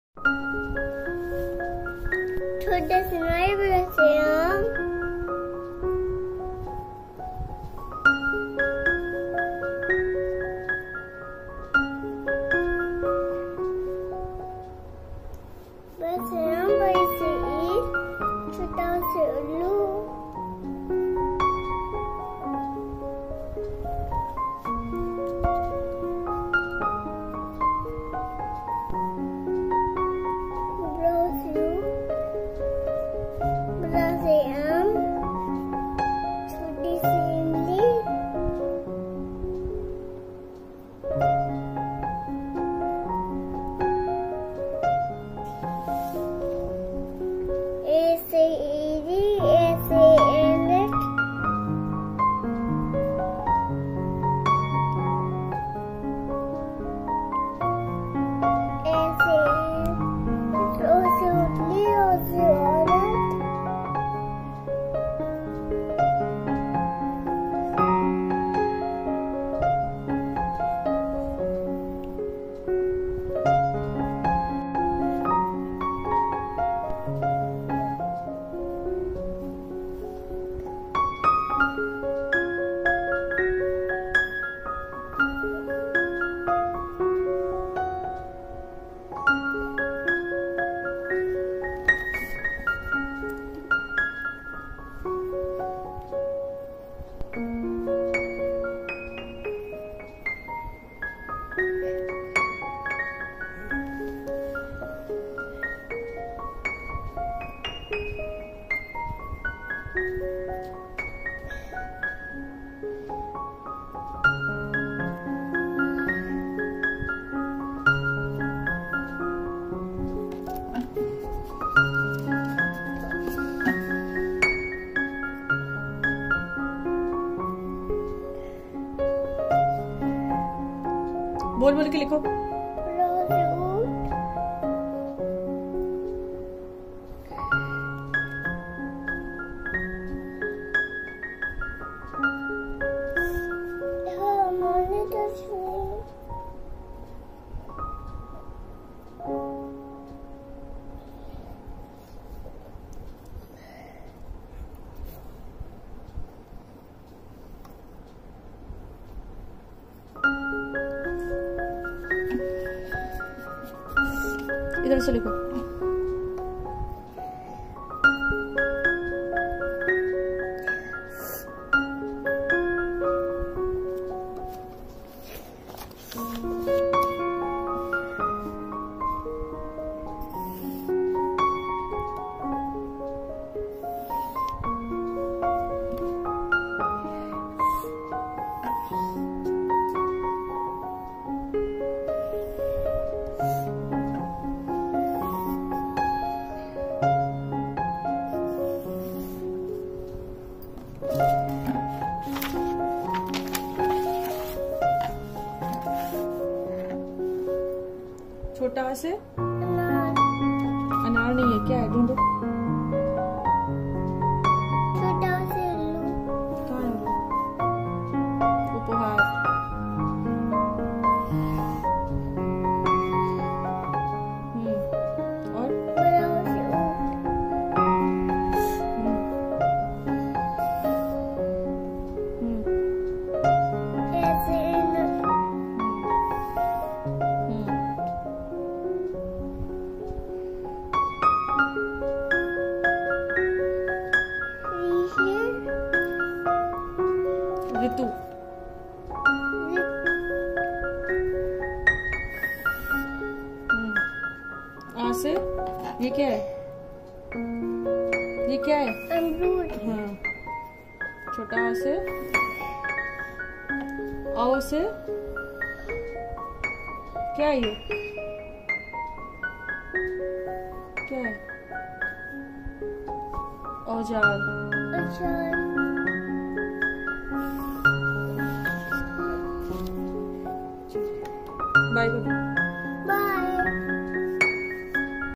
LKG kid practising Hindi worksheet
हिंदी में दो अक्षर के शब्द के बाद क्या पढ़ाएं? तीन अक्षर वाले शब्द कैसे शुरू करें!!